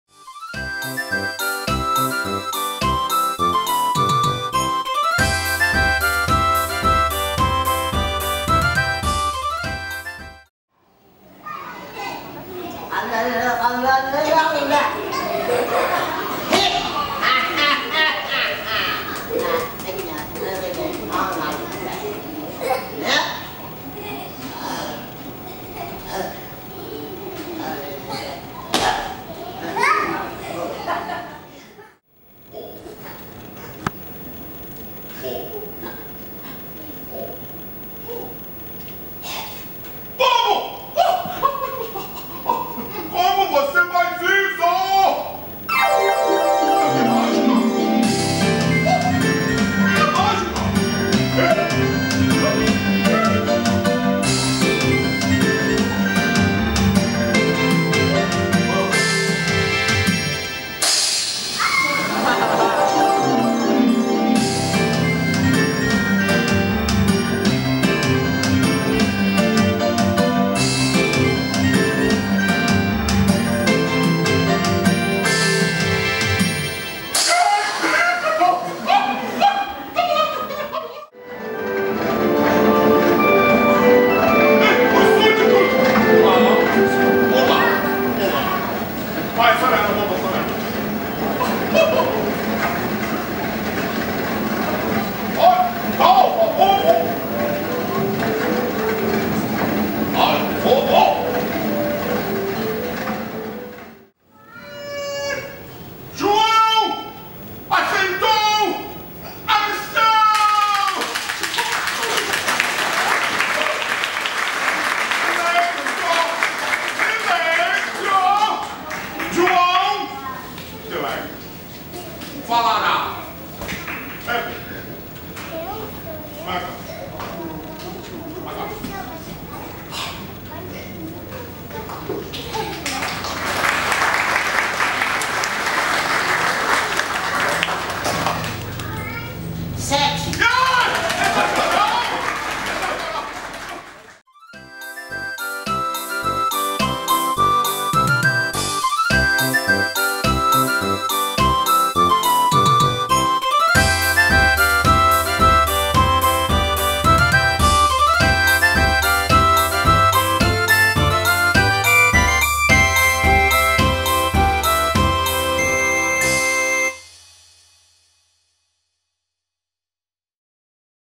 João o Alfaiate - Um herói inusitado - Etc e Tal 1993/2017 ®
Teaser (Trechos do espetáculo) "João o Alfaiate - Um herói inusitado" do 'Centro Teatral e Etc e Tal' , companhia carioca de repertório do trio de cômicos alvaro Assad, Marcio Moura e Melissa Teles-Lôbo, criada em 1993 e especializada em Teatro-Mímica-Humor. Décimo espetáculo de repertório. Adaptação do conto original dos Irmãos Grimm (João Mata 7).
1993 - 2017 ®